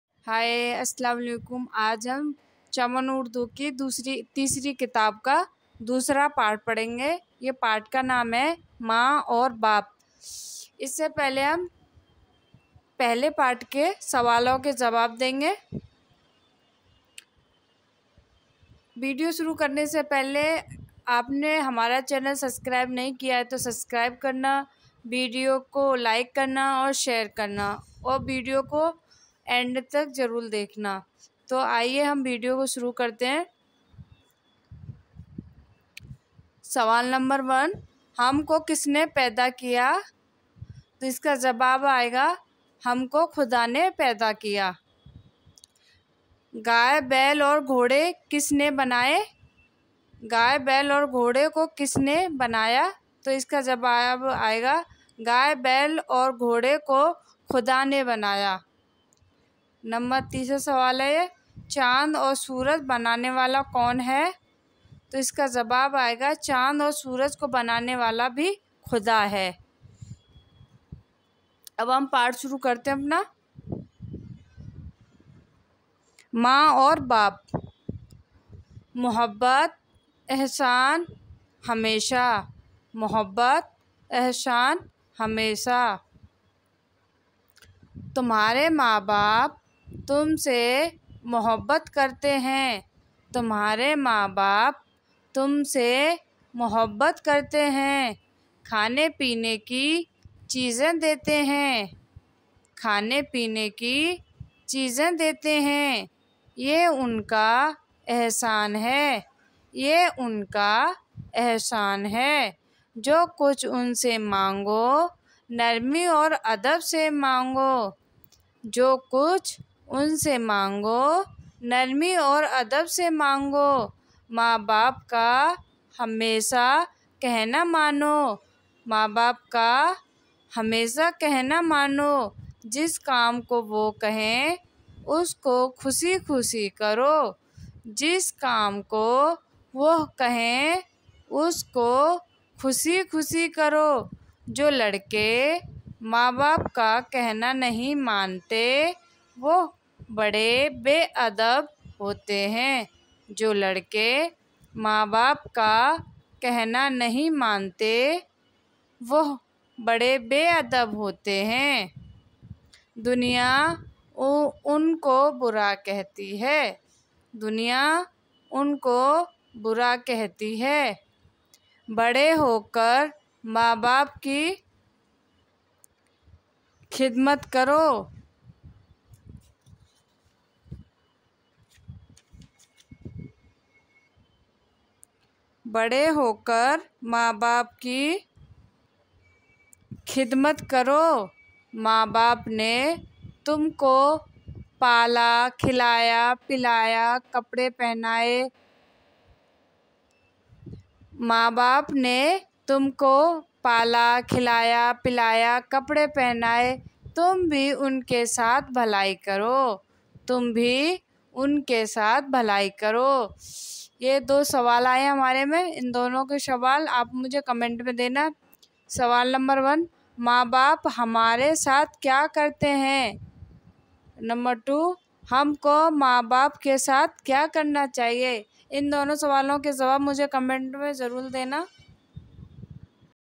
पाठ का नाम मां और बाप | Chaman Urdu ki Teesri kitab Ka sabak number 2 | free mein Urdu padhna sikhen
paath ka naam man aur baap | chamar Urdu ki Teesri kitab Ka sabak number 2 | free mein Urdu padhna sikhen |

पाठ का नाम मां और बाप |चमन उर्दू की तीसरी किताब का सबक नंबर 2  | फ्री में उर्दू पढ़ना सीखें